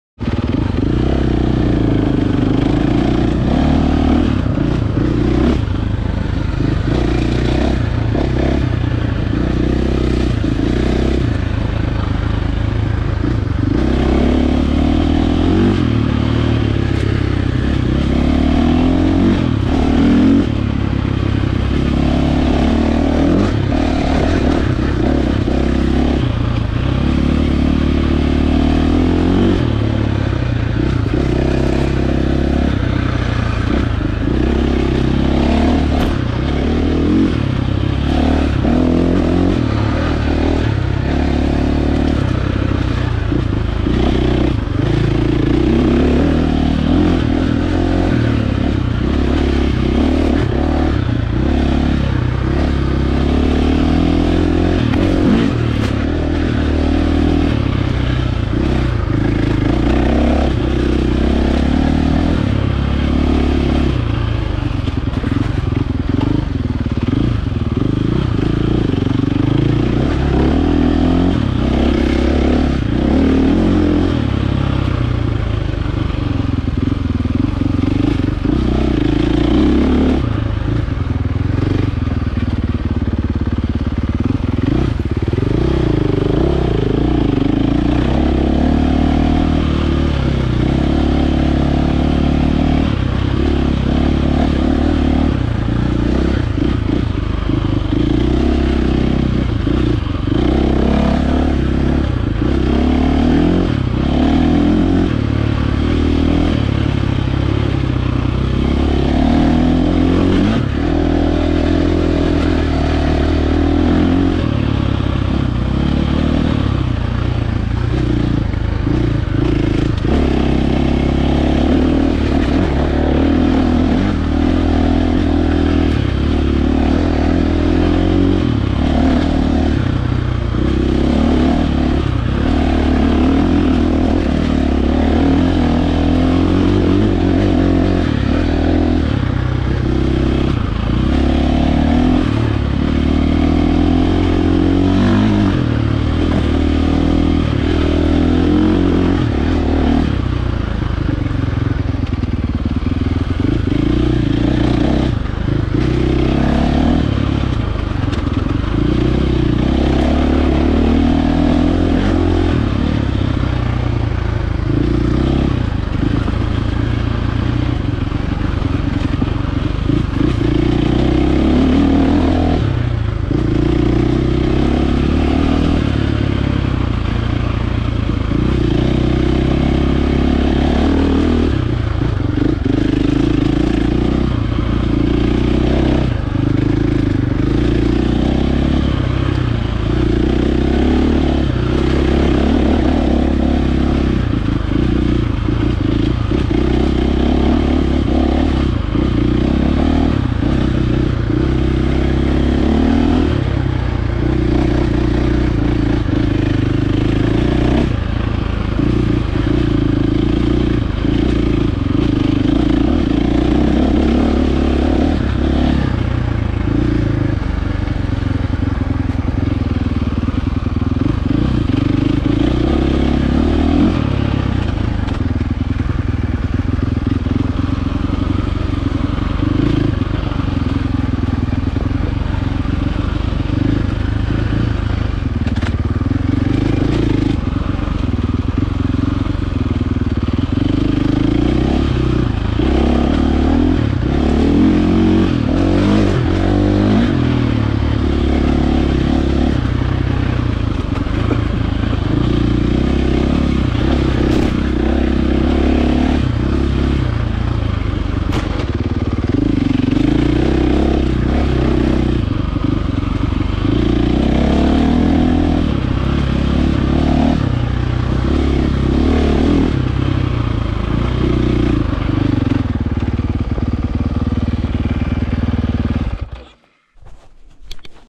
Is a 450 too much bike for riding in the woods?
Tell me what you think is the best bike for riding in the woods, and why?
I currently have this 2023.5 KTM 450SXF FE that's set up for the woods, and I also have a 2022 KTM 250XC TPI with TSP. They're both great for different things, but if you could only have one bike to ride in the woods, what would it be?